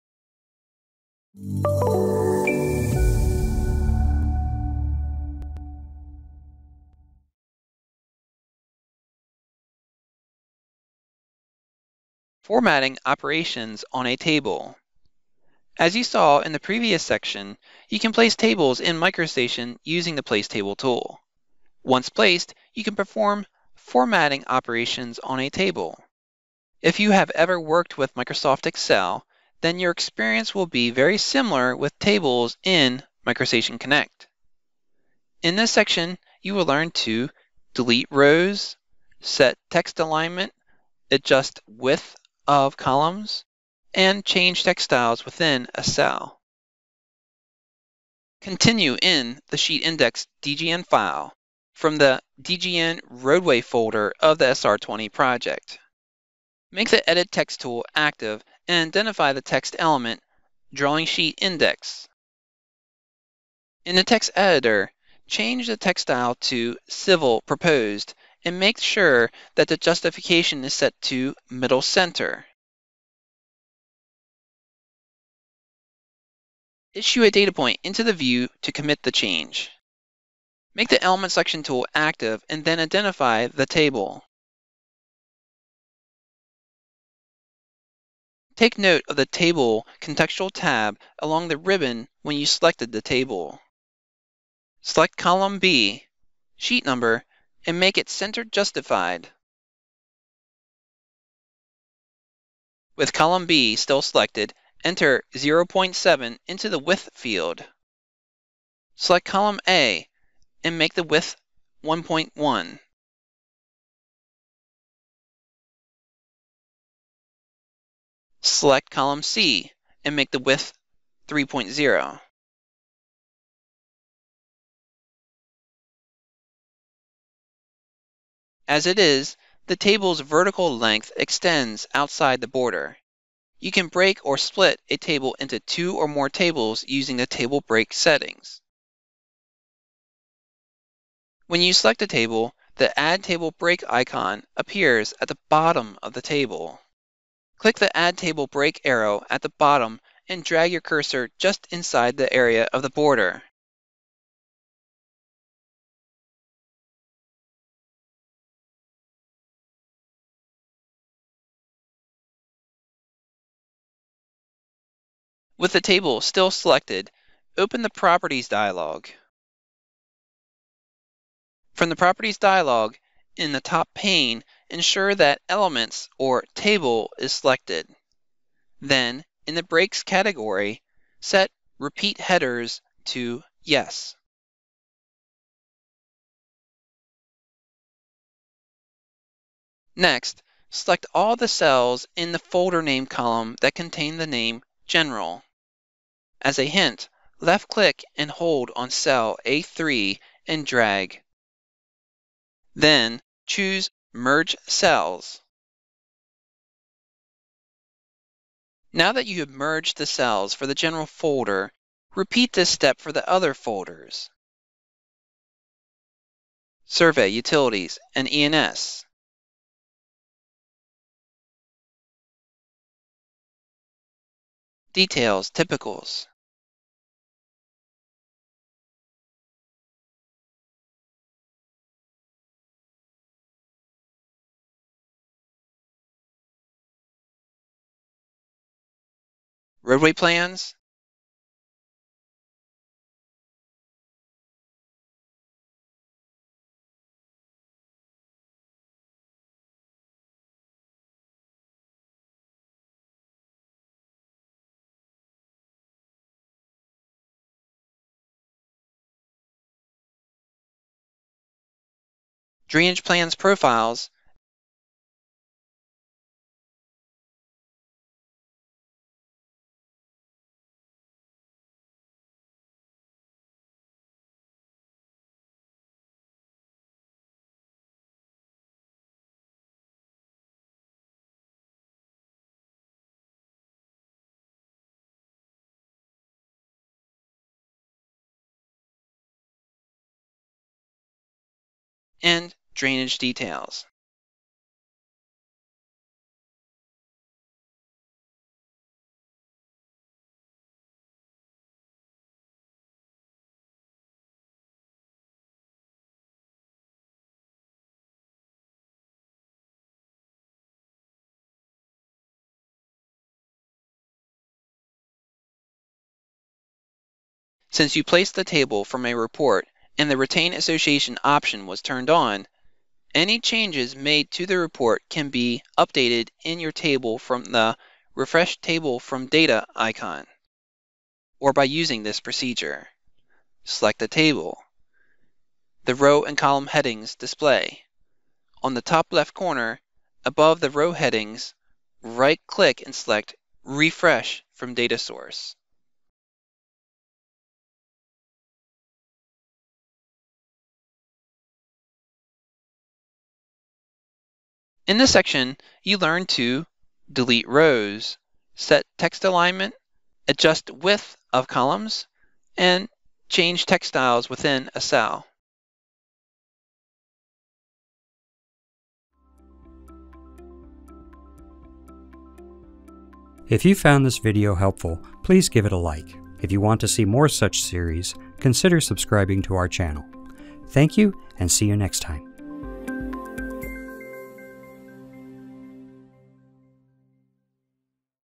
Formatting Operations on a Table
As you saw in the previous section, you can place tables in MicroStation using the Place Table tool. Once placed, you can perform formatting operations on a table. If you ever have worked with Microsoft Excel, then your experience will be very similar with tables in MicroStation CONNECT Edition.